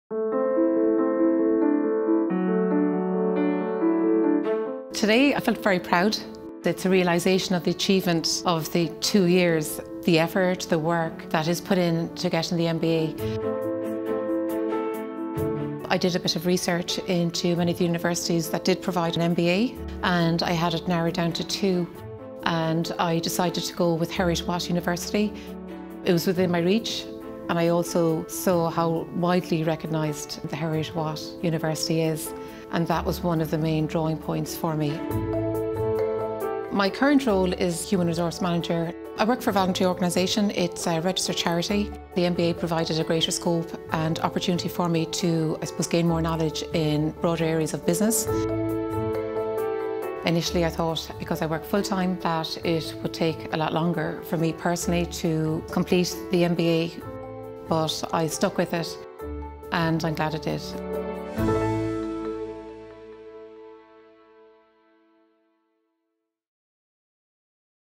Meet Sinead Noonan, MBA graduate from Ireland
Edinburgh Business School MBA graduate Sinead Noonan explains why she chose the Edinburgh Business School MBA programme.

Sinead studied for her MBA in Ireland via distance learning.